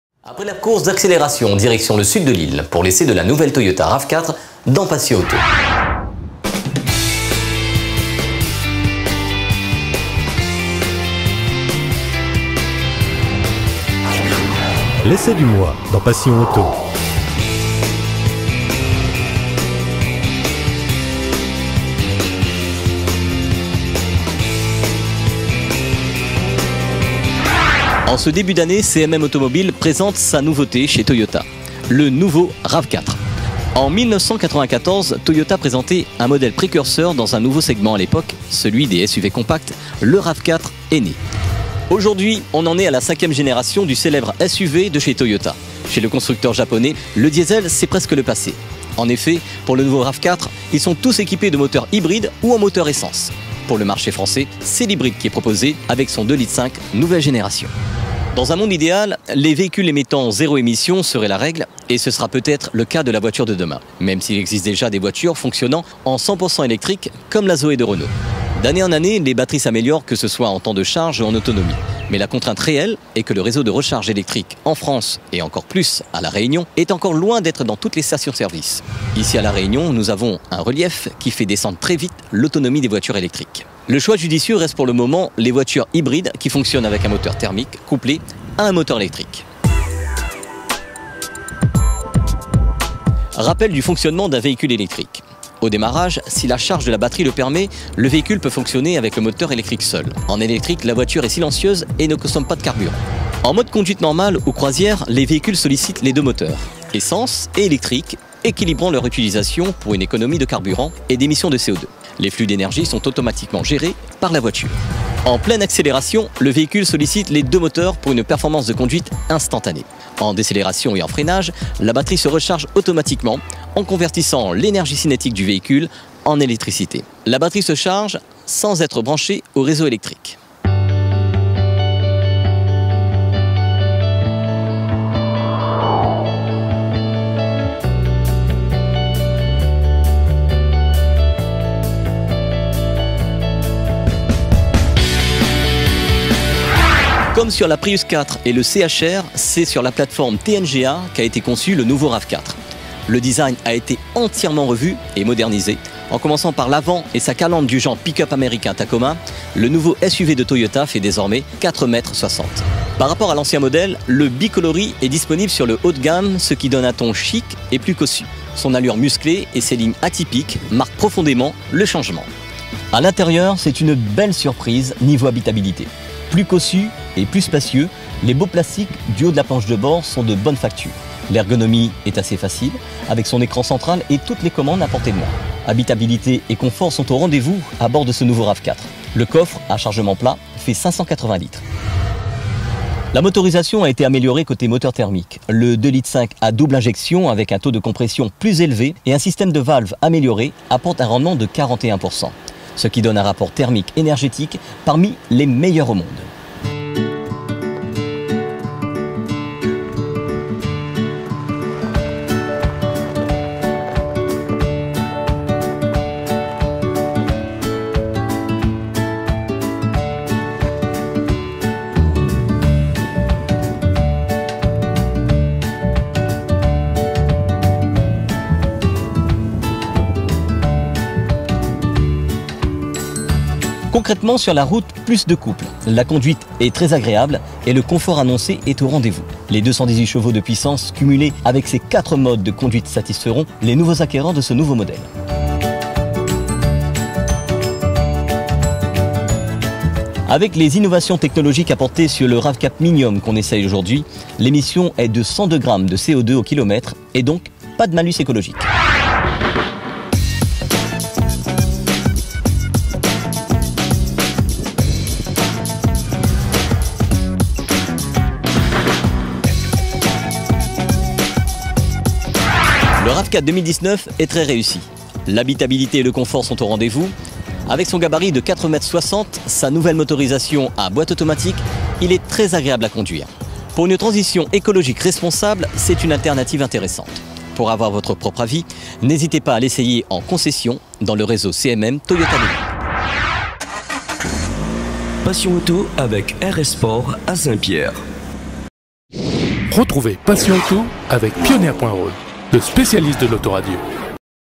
Essai le Nouveau Toyota Rav4 2019 Extrait Passion Auto 207
En 2019 , nous avons essayé pour vous le nouveau Rav4 Hybride équipe du nouveau moteur 2,5l ...passionauto.re ...